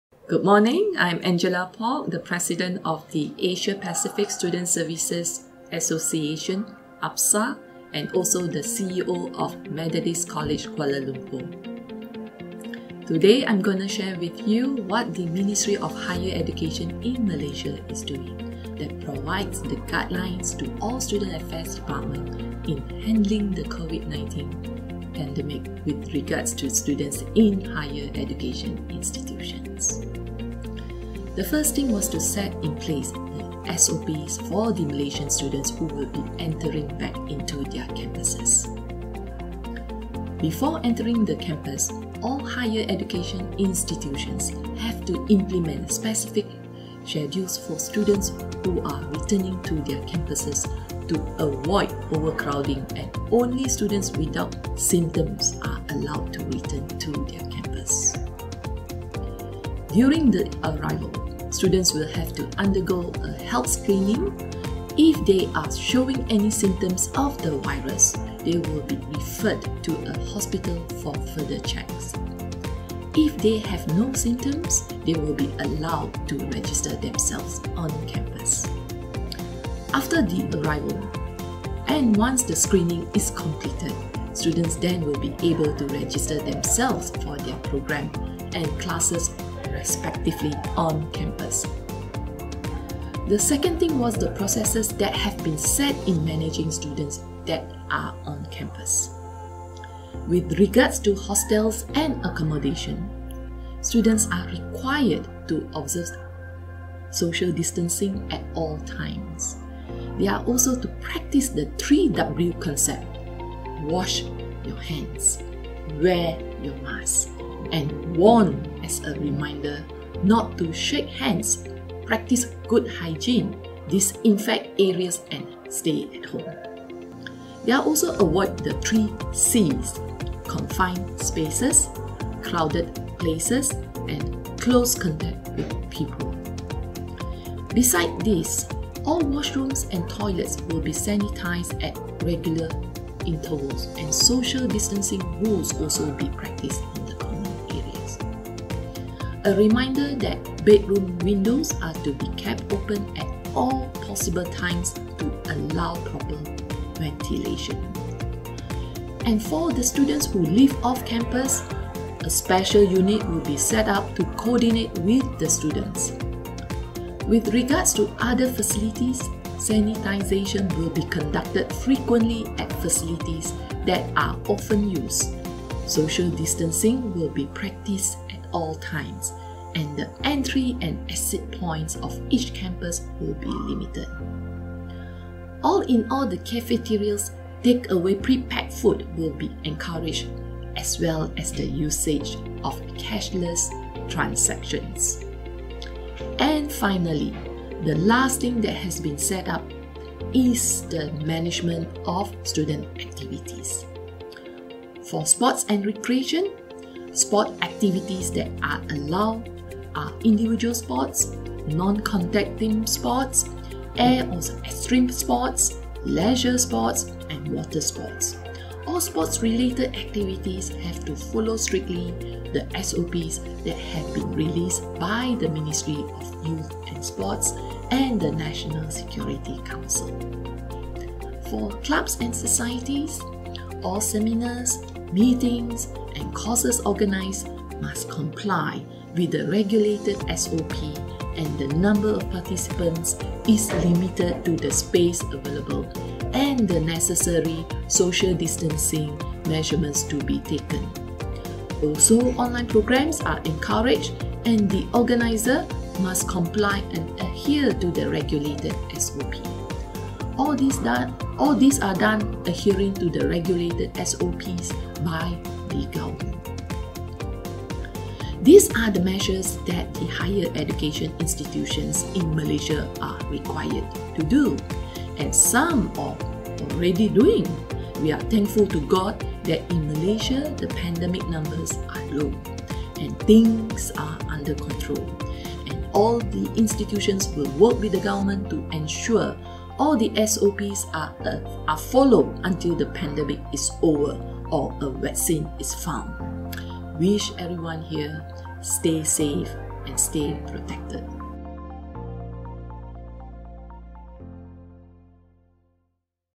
APSSA Webinar 2020: Malaysia Higher Education Institution
As the President of Asia Pacific Student Services Association (APSSA) for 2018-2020, our CEO Ms Angela Pok presents some of the steps taken by Malaysian higher education institution in response to COVID-19.